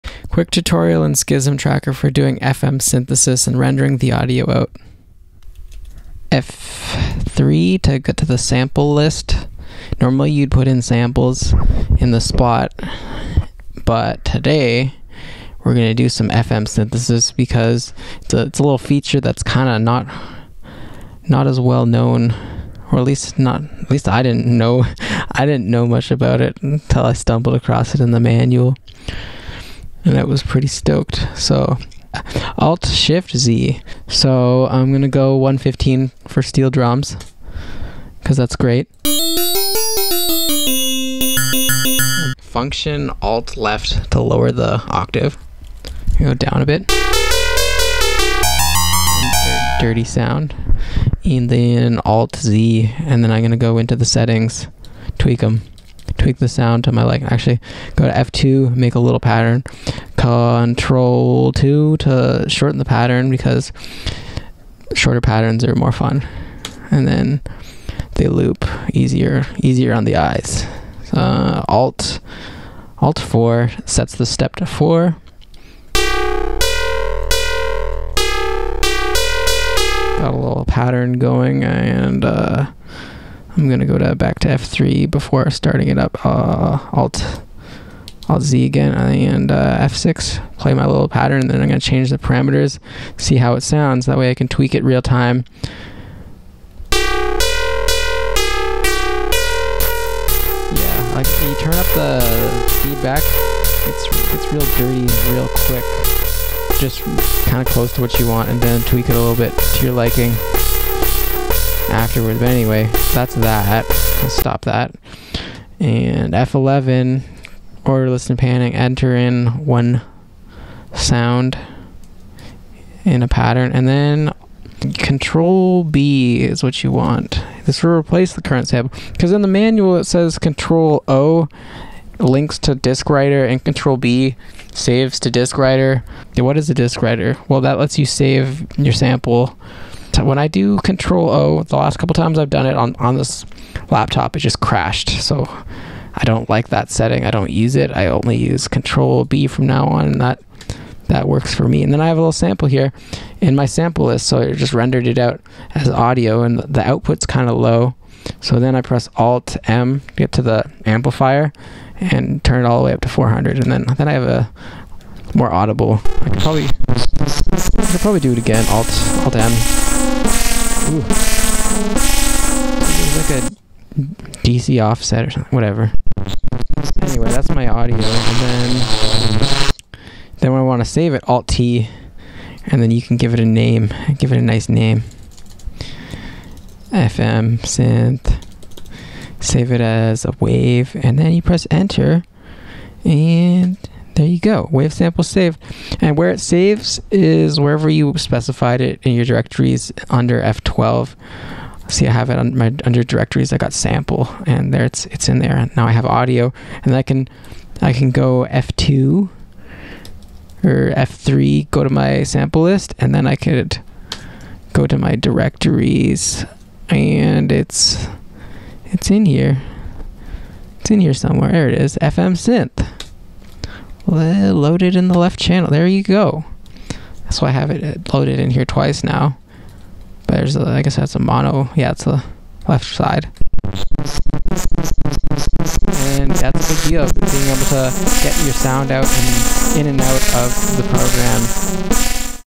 fm synthesis in schism tracker
schismtracker tutorial explaining how to enable general midi fm patches, edit them, and render them to wav